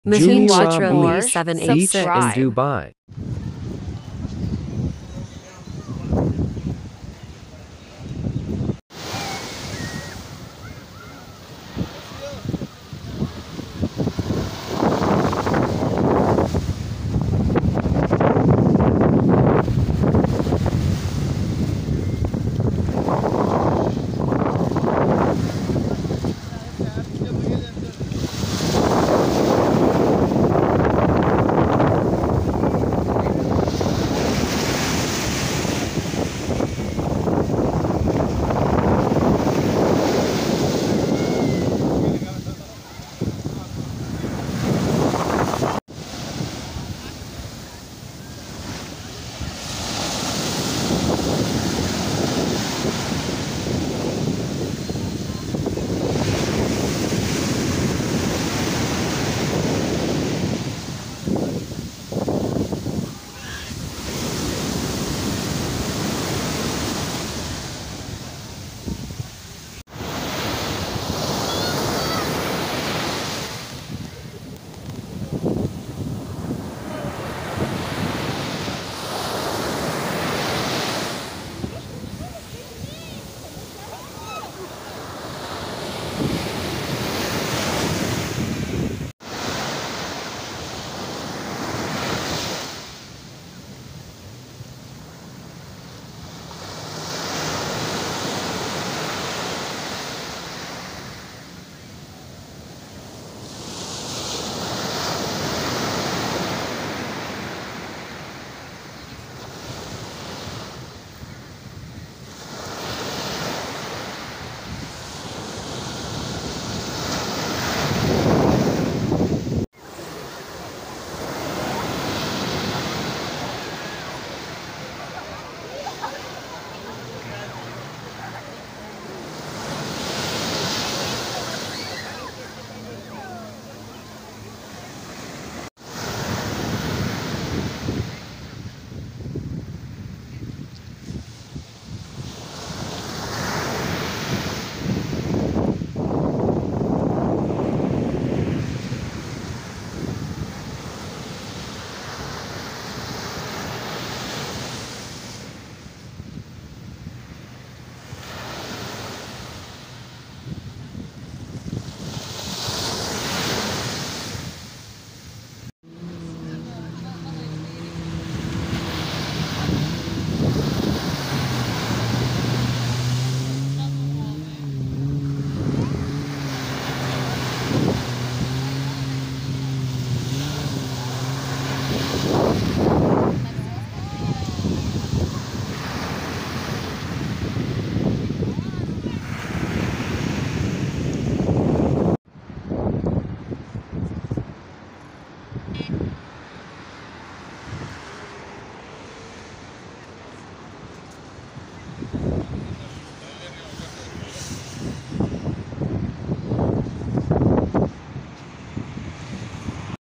Dubai Beach Walk || Jumeirah Beach || Beach In Dubai 2023
Jumeira Beach is a white sand beach that is located and named after the Jumeirah district of Dubai, United Arab Emirates, on the coast of the Persian.